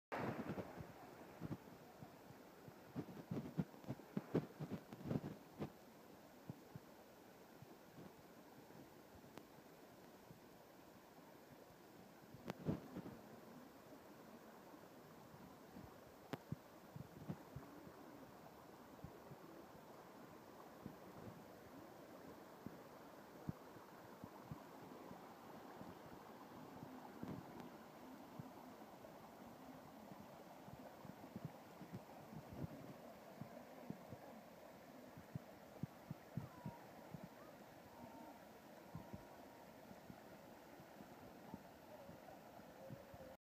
Le Tavignanu, rivière et sentier de randonnée à Corte
Le Tavignanu est une rivière de montagne qui passe notamment par Corte, où les amateurs de sentiers sauvages et d'escales fraîcheur trouveront leur compte.